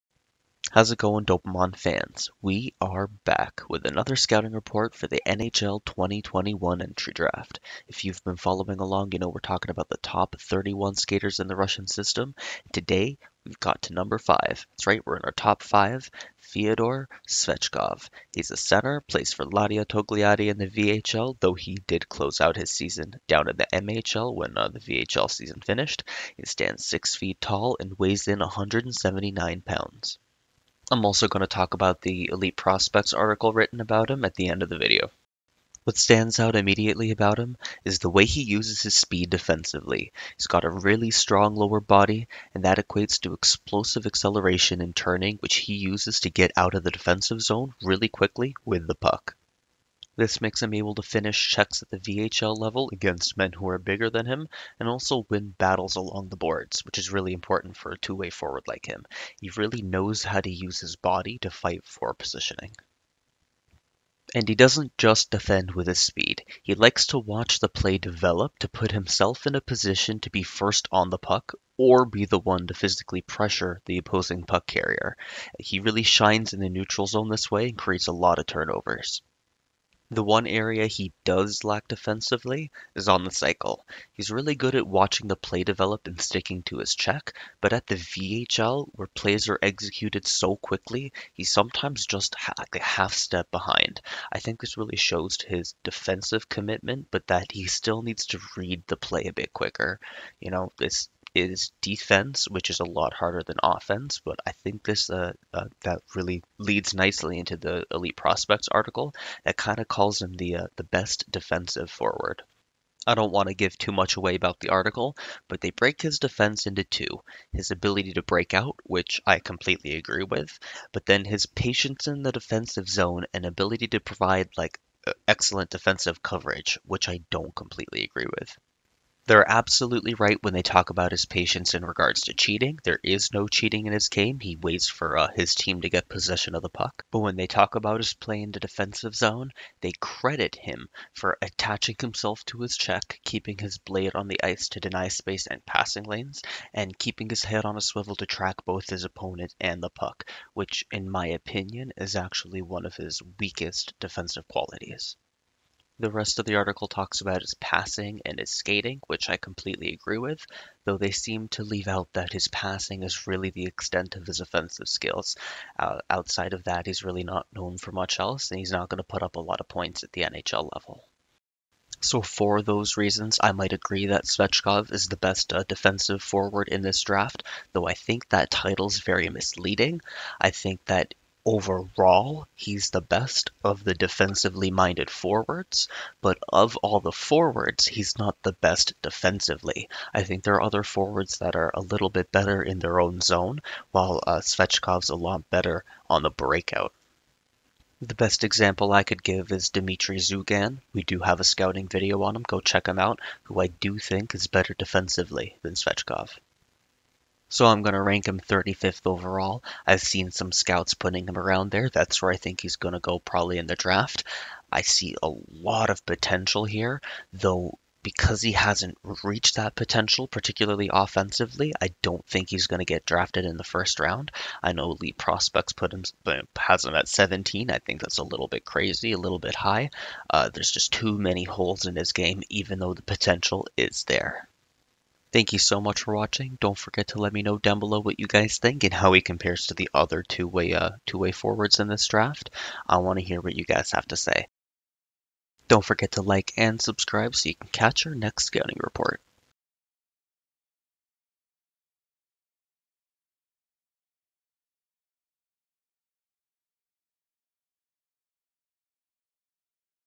Fyodor Svechkov | #5 Top Russian Prospects | 2021 NHL Entry Draft Scouting Report
The Nashville Predators selected Fyodor Svechkov 19th overall in the 2021 NHL Entry Draft

How's it going Dopemon fans? We got our next scouting report for the 2021 NHL Draft today, Fyodor Svechkov, also written as Feder Svechkov, from Ladia Togliatti of the VHL.

We also respond to the EliteProspects article written about him, Fyodor Svechkov just might be the 2021 NHL Draft's best defensive forward: